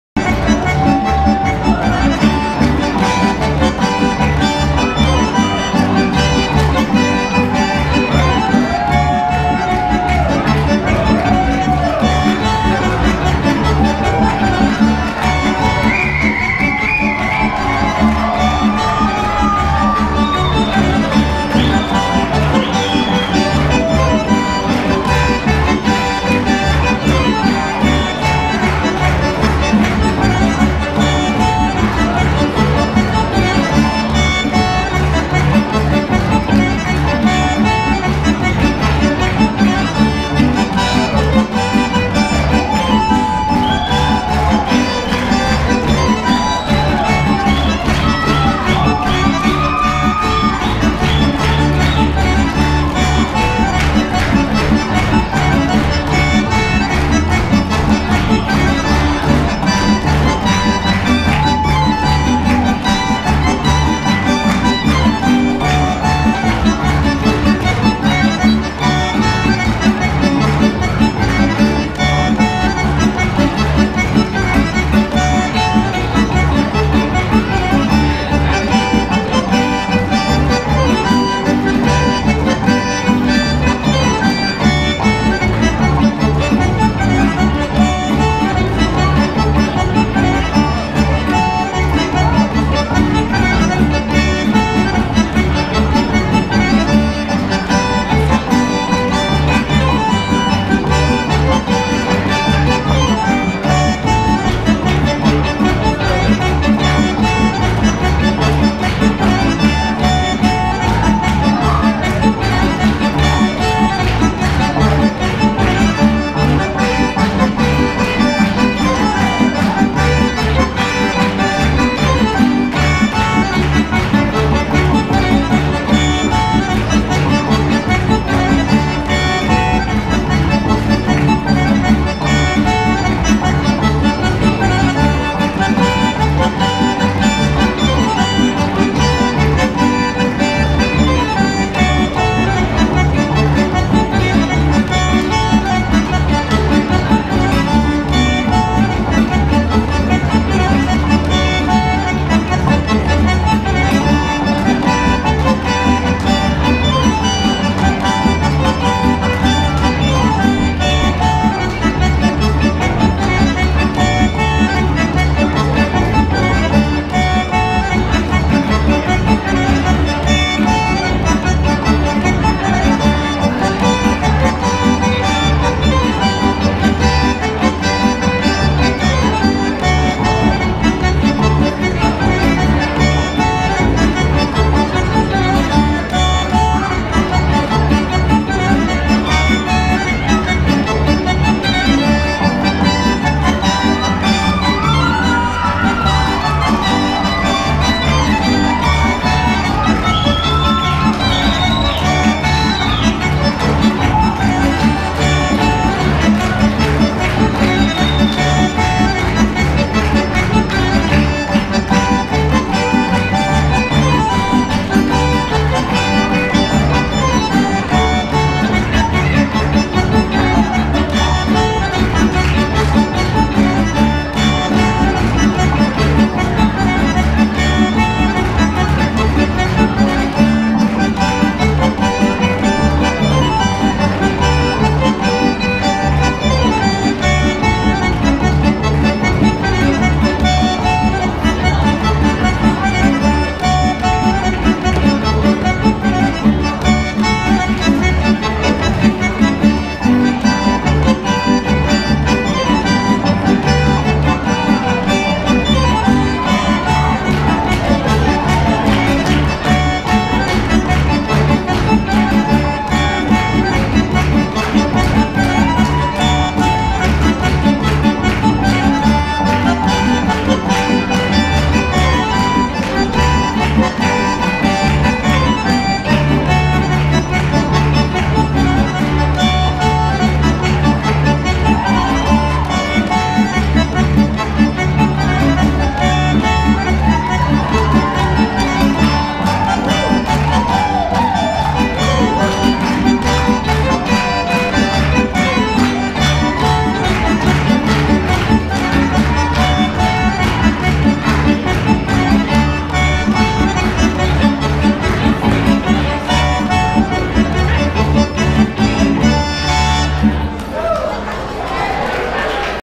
Hopak at the Russian Ball
Alexander Sevastian - Accordion
Albina Molodozhan - Violin
Bill Bridges - Guitar
Colin Maier - Double Bass